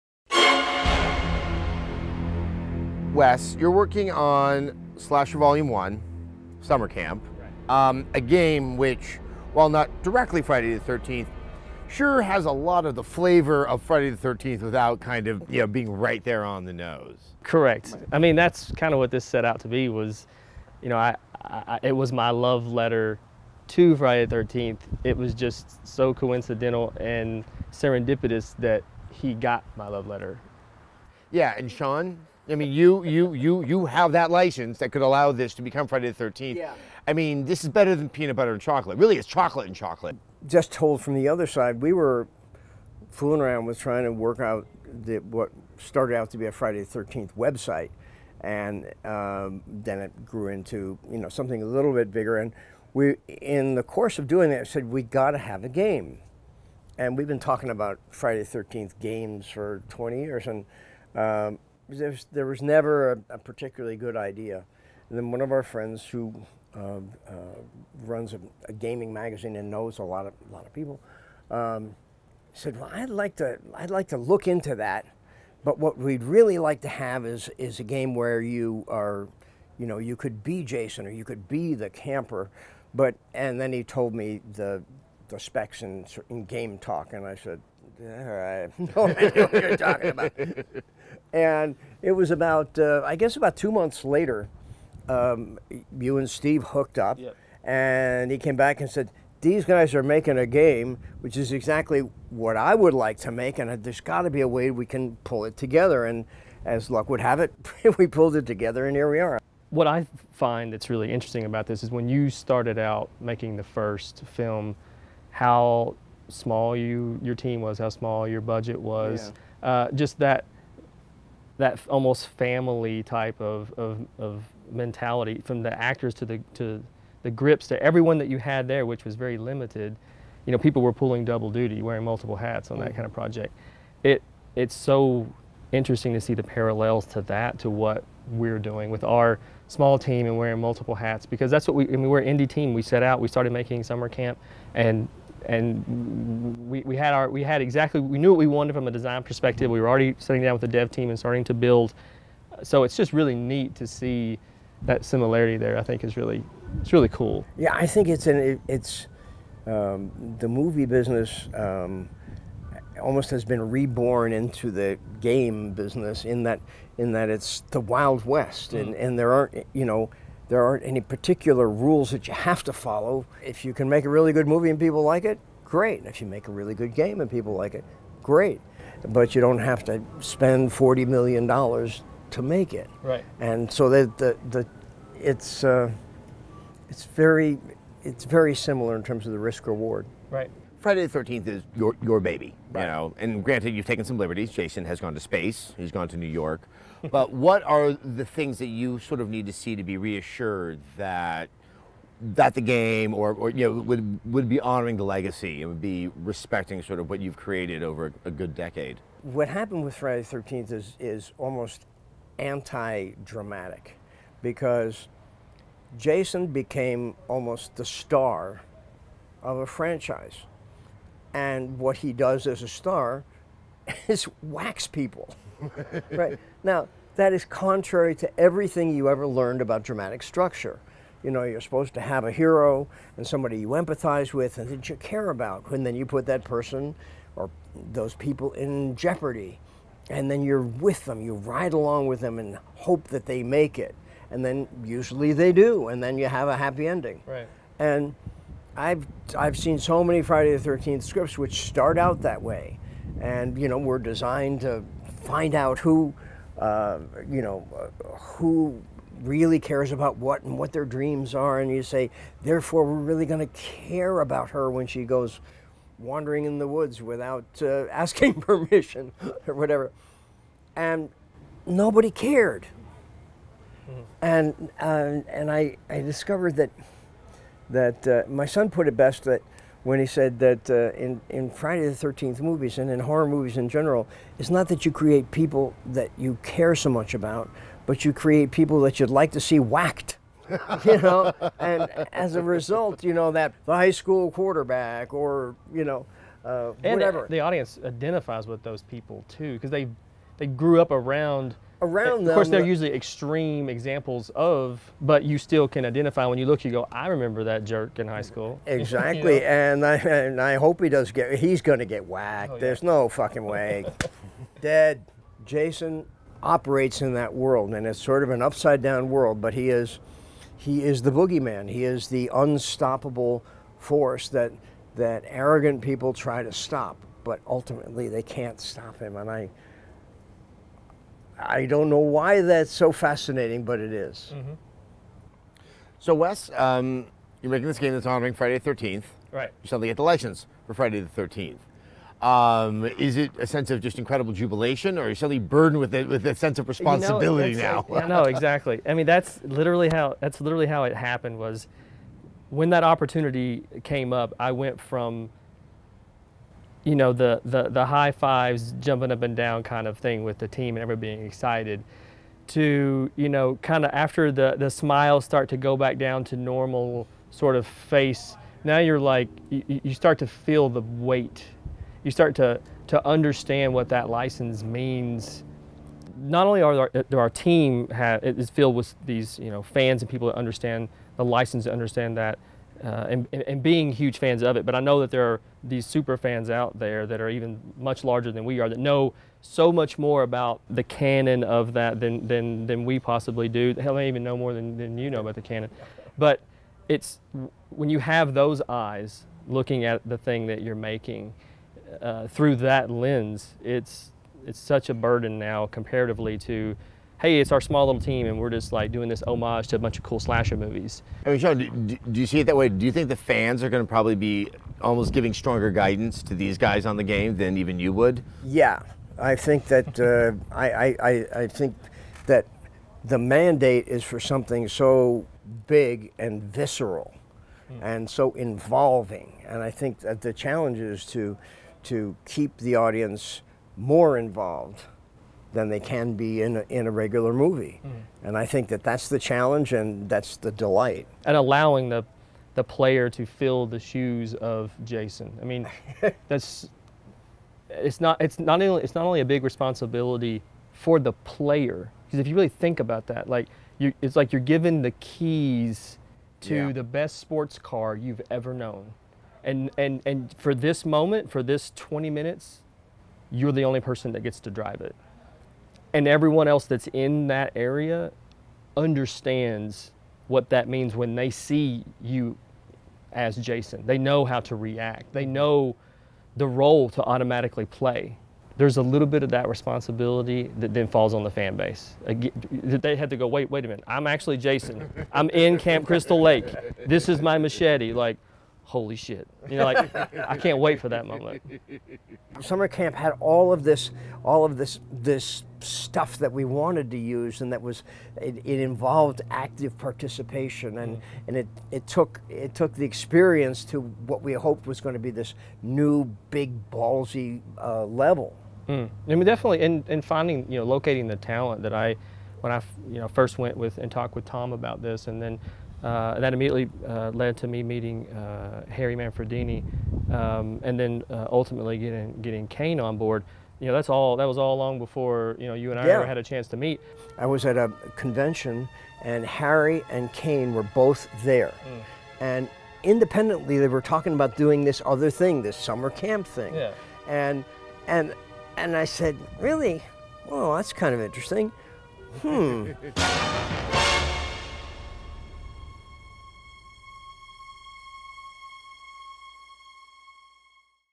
Friday the 13th: The Game - Interview with Sean S. Cunningham and Wes Keltner
Adam Sessler joins Jason Voorhees creator and Friday the 13th film director Sean S. Cunningham and game co-creator Wes Keltner to talk about the challenges of capturing Jason's movements and kills in-game!